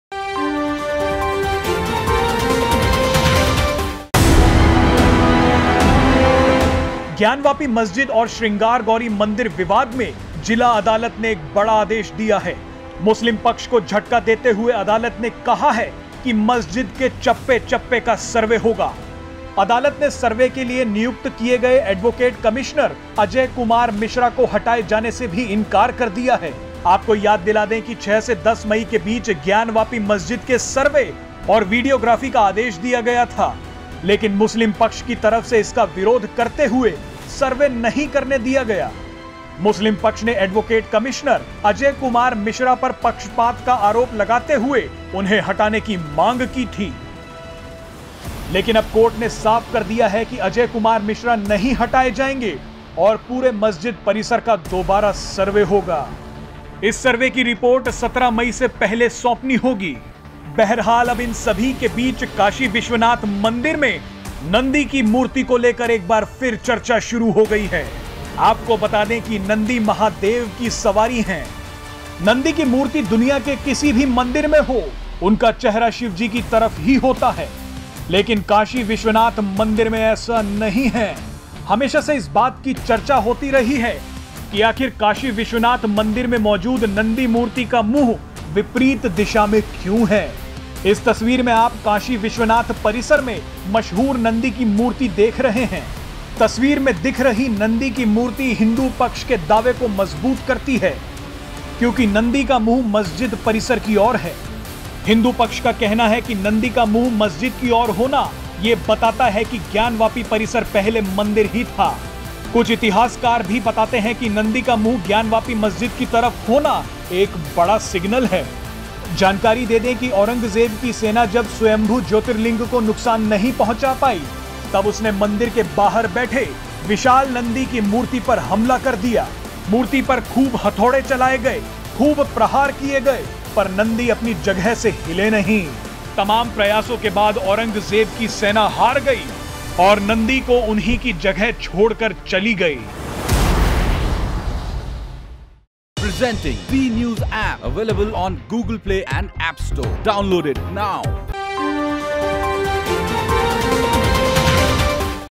Gyanvapi Masjid Controversy: मुस्लिम पक्ष को बड़ा झटका, शिव के नंदी की चर्चा | Kashi |Breaking News
मुस्लिम पक्ष को बड़ा झटका, शिव के नंदी की चर्चा| Gyanvapi Vivaad | Kashi-Vishwanath News| Breaking News

Varanasi court on Thursday, ordered the videographic survey to continue in the Gyanvapi mosque complex while hearing the petition of five women to worship Hindu deities at the disputed site, adjacent to the Kashi-Vishwanath Temple. The court also decided that Ajay Kumar Mishra, the court commissioner for the survey, will continue with his court-mandated duties after a petition filed by the Gyanvapi mosque management committee.

Gyanvapi Masjid Case और श्रृंगार गौरी मंदिर विवाद में जिला अदालत ने बड़ा आदेश दिया है. अदालत ने मुस्लिम पक्ष की कोर्ट कमिश्नर को हटाने की मांग को खारिज कर दिया है. बहरहाल अब इन सभी के बीच कांशी विश्वनाथ मंदिर में स्थित नंदी की मूर्ती को लेकर फिर चर्चा शुरू हो गई है. क्या है ये पूरा मामला जानिए इस वीडियो में...

About Channel: credit - zee news